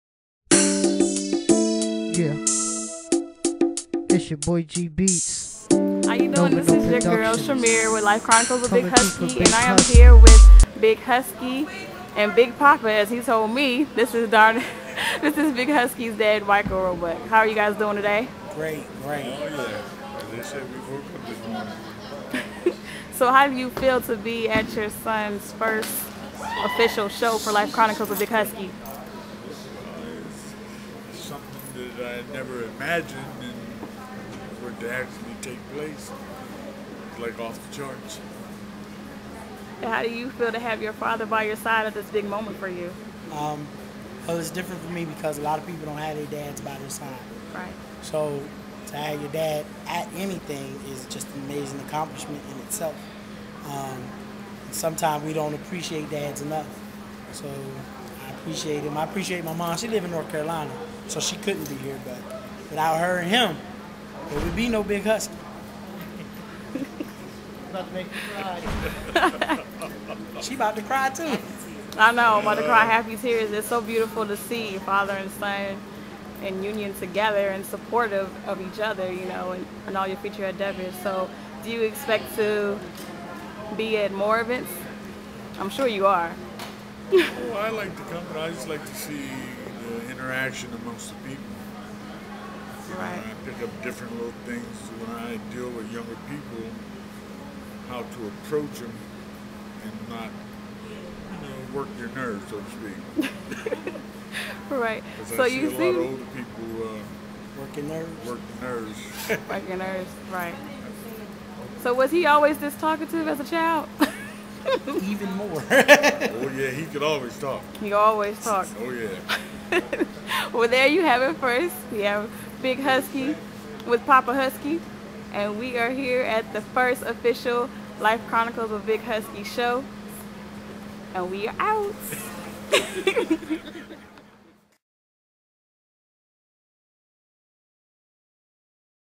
Big Poppa {Michael Roebuck} and Big Husky My Father Is The Best
This is the first time my father allowed himself to be interviewed on camera. I was honored as I sat back and realized that we actually accomplished what we set out to do and that is give the young people something to watch for them by them. I introduce Michael "Big Poppa" Roebuck.